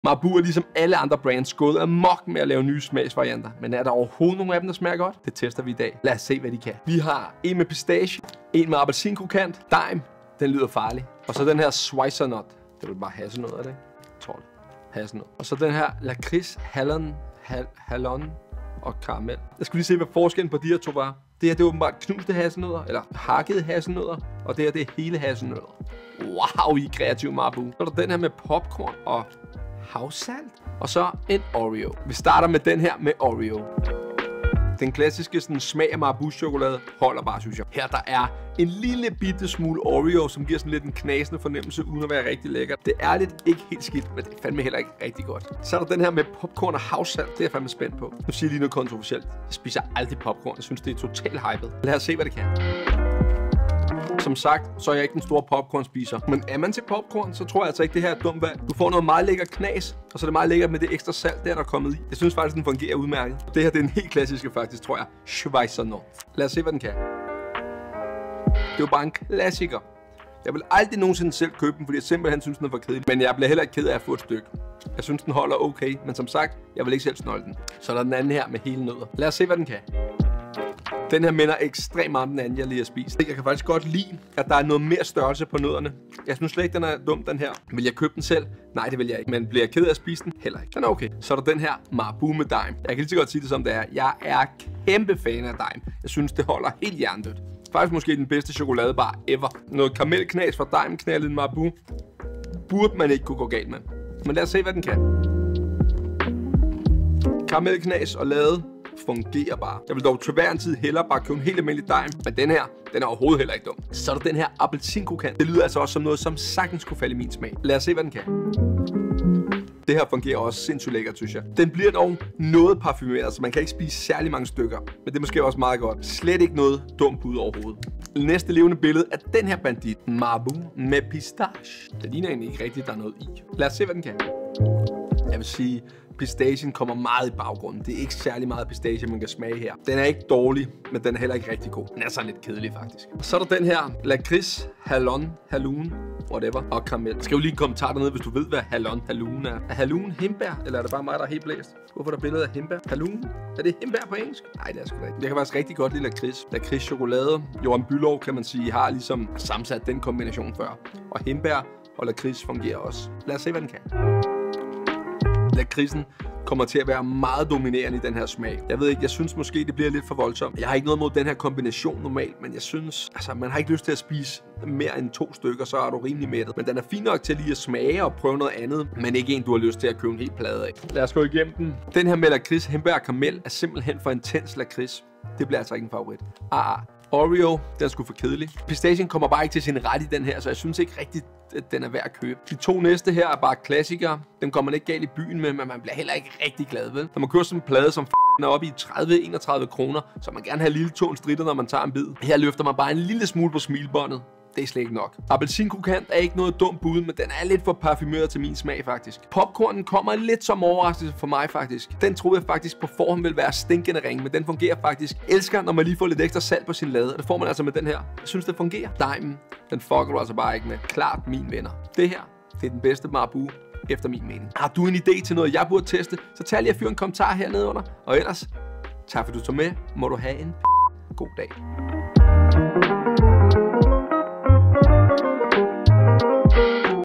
Marabou test - Hvilken en smag er den bedste?
I denne video tester alle de forskellige Marabou smage jeg har kunne finde. Der er de klassiske og nogle helt nye. Hvilken smager starter inde og er lige til at smide ud.